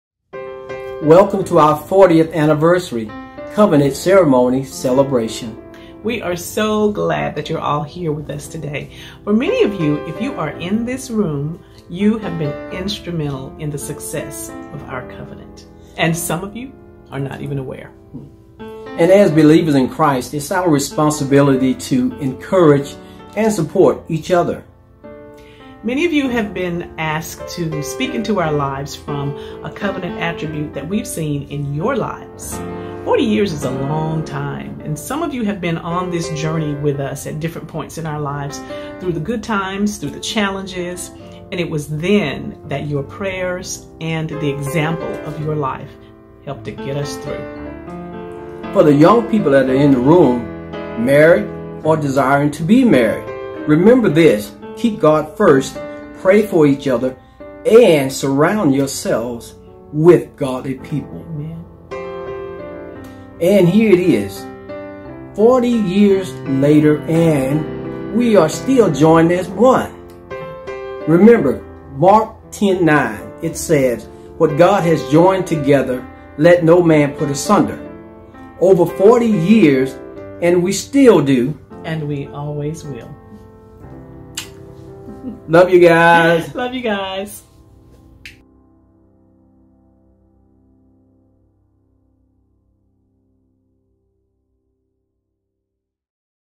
We Still Do ....and We Always Will
This video is about My 40th Wedding Anniversary